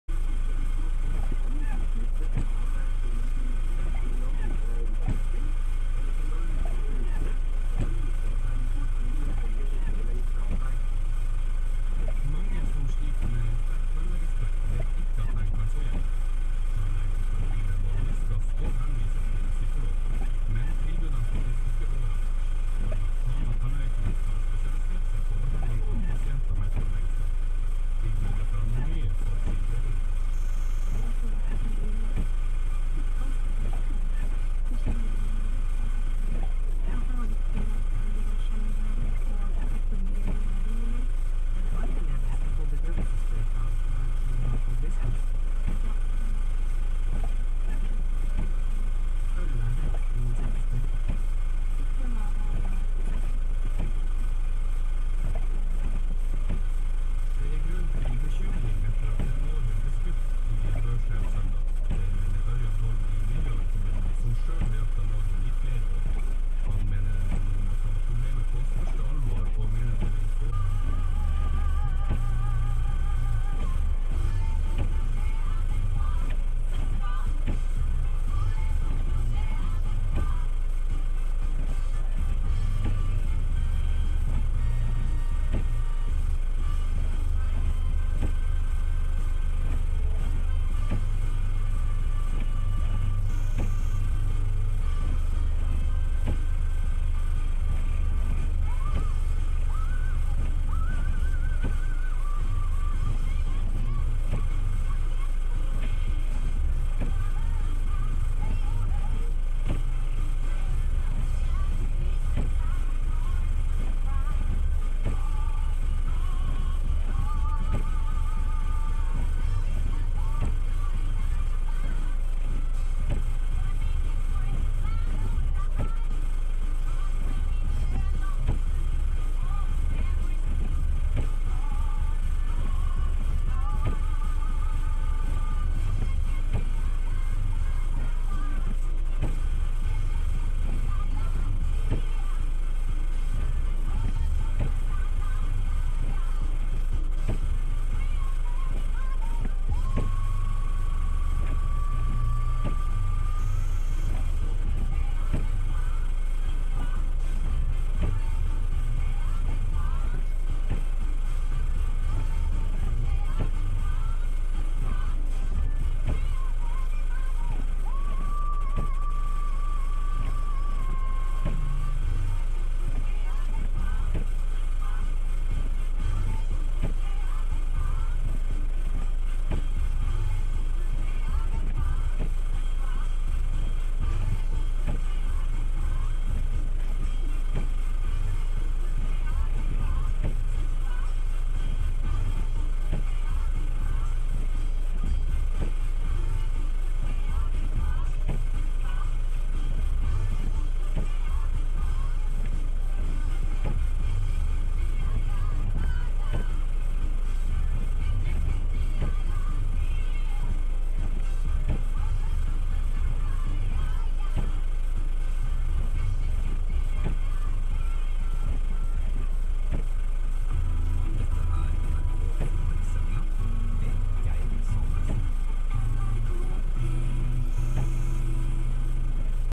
Polaris Sportsman 570 Snow Plowing
Plowing our steep driveway with my new Polaris Sportsman 570